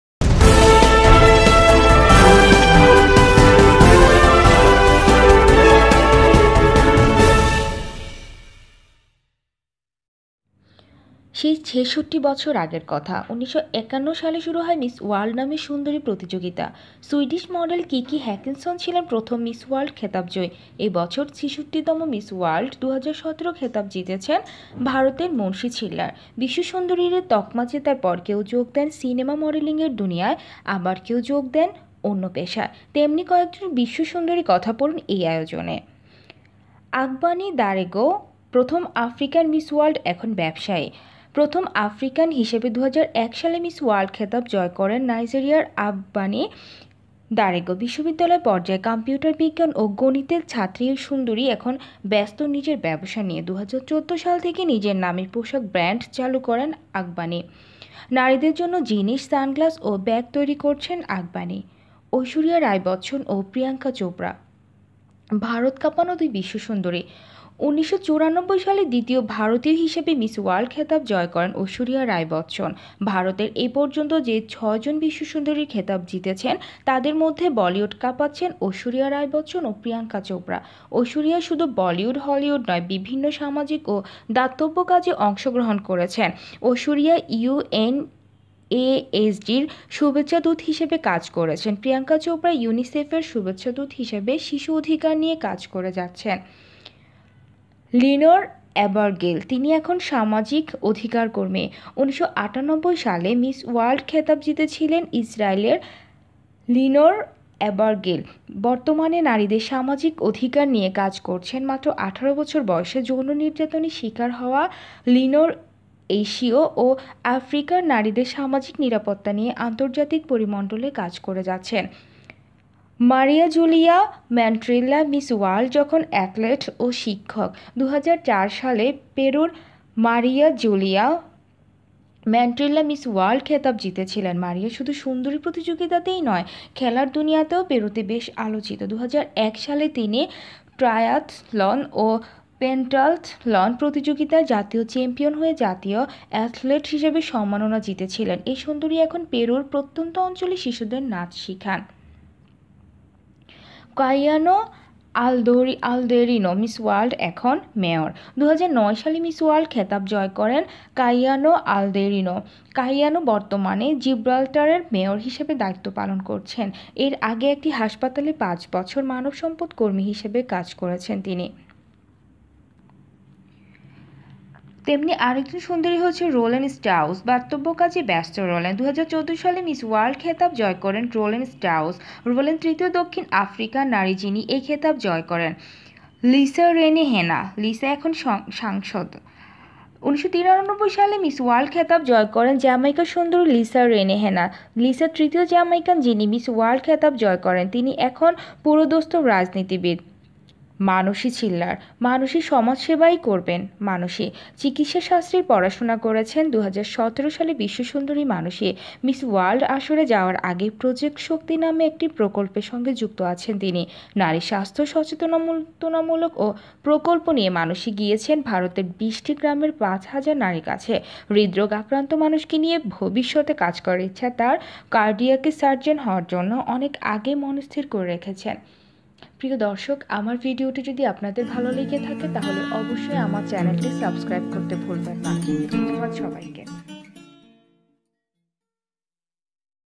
জানলে অবাক হবেন তারকা বিশ্বসুন্দরীরা এখন কে কোথায় | Biography of all Miss World | New Reports 2017
সেই ৬৬ বছর আগে ১৯৫১ সালে শুরু হয় ‘মিস ওয়ার্ল্ড’ নামের সুন্দরী প্রতিযোগিতা। সুইডিশ মডেল কিকি হ্যাকানসন ছিলেন প্রথম মিস ওয়ার্ল্ড খেতাবজয়ী। এ বছর ৬৬তম মিস ওয়ার্ল্ড ২০১৭ খেতাব জিতেছেন ভারতের মানুষি ছিল্লার। বিশ্বসুন্দরীরা তকমা জেতার পর কেউ যোগ দেন সিনেমা-মডেলিংয়ের দুনিয়ায়, কেউবা যোগ দেন অন্য পেশায়। তেমনি কয়েকজন বিশ্বসুন্দরীর কথা পড়ুন এই আয়োজনে

##  আগবানি দারেগো
প্রথম আফ্রিকান মিস ওয়ার্ল্ড এখন ব্যবসায়ী -
প্রথম আফ্রিকান হিসেবে ২০০১ সালে মিস ওয়ার্ল্ড খেতাব জয় করেন নাইজেরিয়ার আগবানি দারেগো। বিশ্ববিদ্যালয় পর্যায়ে কম্পিউটারবিজ্ঞান ও গণিতের ছাত্রী এই সুন্দরী এখন ব্যস্ত নিজের ব্যবসা নিয়ে। ২০১৪ সাল থেকে নিজের নামে পোশাক ব্র্যান্ড চালু করেন আগবানি। নারীদের জন্য জিনস, সানগ্লাস ও ব্যাগ তৈরি করছেন আগবানি।

 ## ঐশ্বরিয়া রাই বচ্চন ও প্রিয়াঙ্কা চোপড়া
ভারত-কাঁপানো দুই বিশ্ব সুন্দরী -
১৯৯৪ সালে দ্বিতীয় ভারতীয় হিসেবে মিস ওয়ার্ল্ড খেতাব জয় করেন ঐশ্বরিয়া রাই বচ্চন। ভারতের এ পর্যন্ত যে ছয়জন বিশ্বসুন্দরী খেতাব জিতেছেন, তাঁদের মধ্যে বলিউড কাঁপাচ্ছেন ঐশ্বরিয়া রাই বচ্চন ও প্রিয়াঙ্কা চোপড়া। ঐশ্বরিয়া শুধু বলিউড–হলিউড নয়, বিভিন্ন সামাজিক ও দাতব্য কাজে অংশগ্রহণ করছেন। ঐশ্বরিয়া ইউএনএইডসের শুভেচ্ছাদূত হিসেবে কাজ করছেন। প্রিয়াঙ্কা চোপড়া ইউনিসেফের শুভেচ্ছাদূত হিসেবে শিশু অধিকার নিয়ে কাজ করে যাচ্ছেন।

## লিনর অ্যাবারগিল
তিনি এখন সামাজিক অধিকারকর্মী
১৯৯৮ সালে মিস ওয়ার্ল্ড খেতাব জিতেছিলেন ইসরায়েলের লিনর অ্যাবারগিল। বর্তমানে নারীদের সামাজিক অধিকার নিয়ে কাজ করছেন। মাত্র ১৮ বছর বয়সে যৌন নির্যাতনের শিকার হওয়া লিনর এশীয় ও আফ্রিকার নারীদের সামাজিক নিরাপত্তা নিয়ে আন্তর্জাতিক পরিমণ্ডলে কাজ করে যাচ্ছেন।

   ## মারিয়া জুলিয়া ম্যানটিল্লা
মিস ওয়ার্ল্ড যখন অ্যাথলেট ও শিক্ষক
২০০৪ সালে পেরুর মারিয়া জুলিয়া ম্যানটিল্লা মিস ওয়ার্ল্ড খেতাব জিতেছিলেন। মারিয়া শুধু সুন্দরী প্রতিযোগিতাতেই নয়, খেলা-দুনিয়াতে পেরুতে বেশ আলোচিত। ২০০১ সালে তিনি ট্রায়াথলন ও পেন্টাথলন প্রতিযোগিতায় জাতীয় চ্যাম্পিয়ন হয়ে জাতীয় অ্যাথলেট হিসেবে সম্মাননা জিতেছিলেন। এই সুন্দরী এখন পেরুর প্রত্যন্ত অঞ্চলের শিশুদের নাচ শেখান।

## কাইয়ানো অলদোরিনো
মিস ওয়ার্ল্ড এখন মেয়র!
২০০৯ সালে মিস ওয়ার্ল্ড খেতাব জয় করেন কাইয়ানো অলদোরিনো। কাইয়ানো বর্তমানে জিব্রাল্টারের মেয়র হিসেবে দায়িত্ব পালন করছেন। এর আগে একটি হাসপাতালে পাঁচ বছর মানবসম্পদকর্মী হিসেবে কাজ করেছেন তিনি।

## রোলেন স্ট্রাউস
দাতব্য কাজে ব্যস্ত রোলেন
২০১৪ সালে মিস ওয়ার্ল্ড খেতাব জয় করেন রোলেন স্ট্রাউস। রোলেন তৃতীয় দক্ষিণ আফ্রিকার নারী, যিনি এ খেতাব জয় করেন। স্ট্রাউস দক্ষিণ আফ্রিকায় দাতব্য প্রতিষ্ঠান স্ট্রাউস ফাউন্ডেশনের চেয়ারপারসন হিসেবে কাজ করছেন। তাঁর মা ছিলেন নার্স আর বাবা পেশায় চিকিৎসক, তাই মা-বাবার পথ ধরেই মানবসেবার জন্য দাতব্য প্রতিষ্ঠানের সঙ্গে অনেক দিন ধরেই যুক্ত রোলেন।

## লিসা রেনে হ্যানা
লিসা এখন সাংসদ!
১৯৯৩ সালে মিস ওয়ার্ল্ড খেতাব জয় করেন জ্যামাইকান সুন্দরী লিসা রেনে হ্যানা। লিসা তৃতীয় জ্যামাইকান, যিনি মিস ওয়ার্ল্ড খেতাব জয় করেন। তিনি এখন পুরোদস্তুর রাজনীতিবিদ। জ্যামাইকার সংসদীয় ইতিহাসে লিসা সর্বকনিষ্ঠ নারী সাংসদ। ২০১২-১৬ সময়ে তিনি জ্যামাইকার যুব ও সংস্কৃতিবিষয়ক মন্ত্রী হিসেবে দায়িত্ব পালন করেন।

## মানুষি
সমাজসেবাই করবেন মানুষি
চিকিৎসাশাস্ত্রে পড়াশোনা করছেন ২০১৭ সালের বিশ্বসুন্দরী মানুষি। ‘মিস ওয়ার্ল্ড’ আসরে যাওয়ার আগেই ‘প্রজেক্ট শক্তি’ নামের একটি প্রকল্পের সঙ্গে যুক্ত আছেন তিনি। নারীর স্বাস্থ্য সচেতনতামূলক এ প্রকল্প নিয়ে মানুষি গিয়েছেন ভারতের ২০টি গ্রামের পাঁচ হাজার নারীর কাছে। হৃদ্‌রোগে আক্রান্ত মানুষকে নিয়েও ভবিষ্যতে কাজ করার ইচ্ছা তাঁর। কার্ডিয়াক সার্জন হওয়ার জন্য অনেক আগেই মনস্থির করে রেখেছেন।